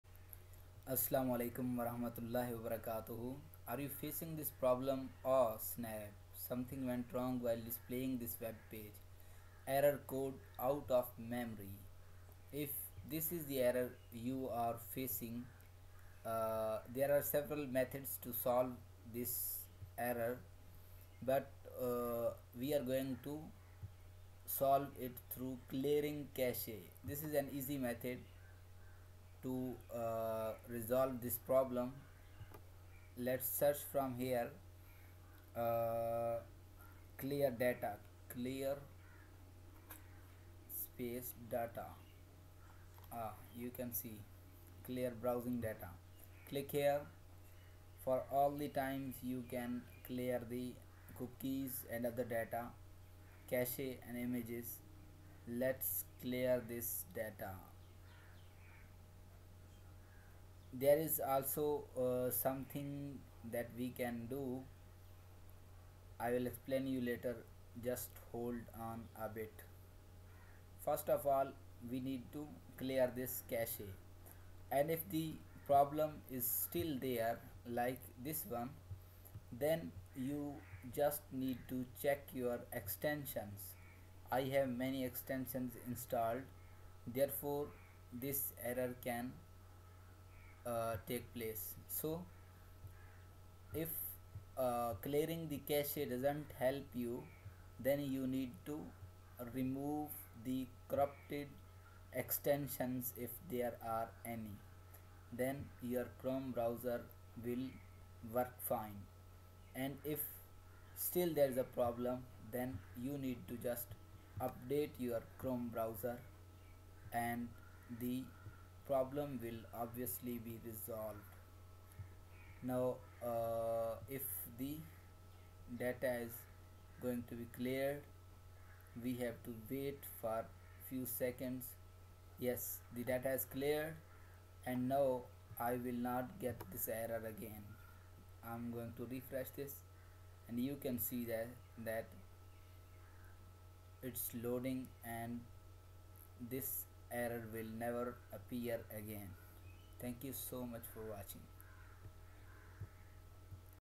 Aw, Snap! Error Code: Out Of Memory in Chrome Browser || FIX 100% Working!!
One of the reasons why you are running out of memory in your Google Chrome browser is when too much temporary data are stored in your cache. ... Select More tools and click Clear browsing data. You can also press Ctrl + Shift + Del all at the same time to go directly to the clear cache data from your main browser screen.
Here are the steps how to solve Aw, Snap! Error code out of memory problem in Chrome browser.
Step 1: Go to settings in chrome.
Step 2: Search for clear data/cache.
Step 3: Clear Cache and Data for browser.
Step 4: Update Browser if needed.
Step 5: Delete it if any extension is Corrupt.
Done!